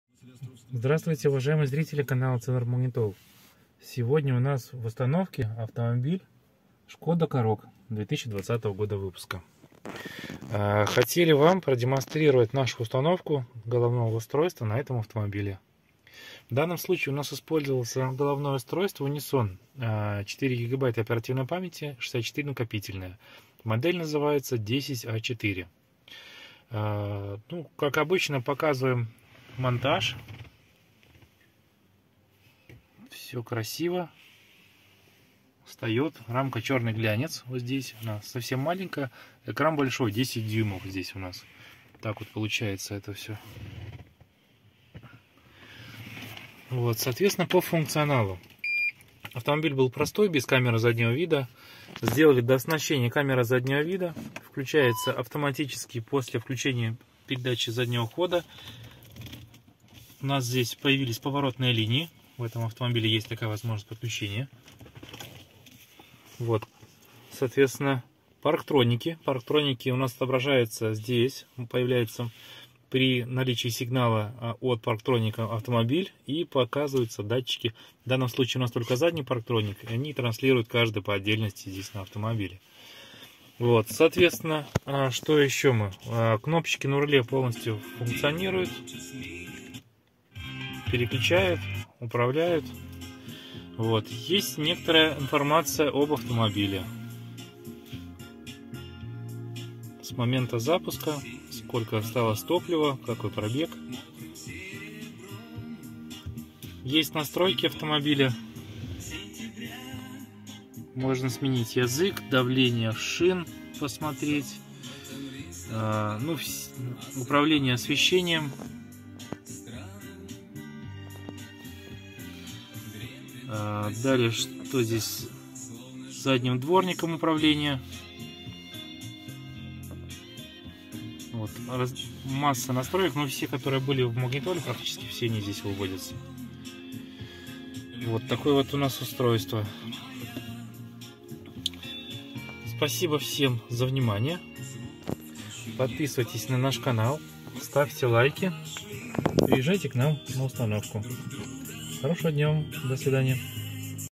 SKODA KAROQ Магнитола UNISON 4/64Gb Sim4G QLED 1280/720 Установка КАМЕРЫ
ПРОДАЖА И УСТАНОВКА
МАГНИТОЛ UNISON А400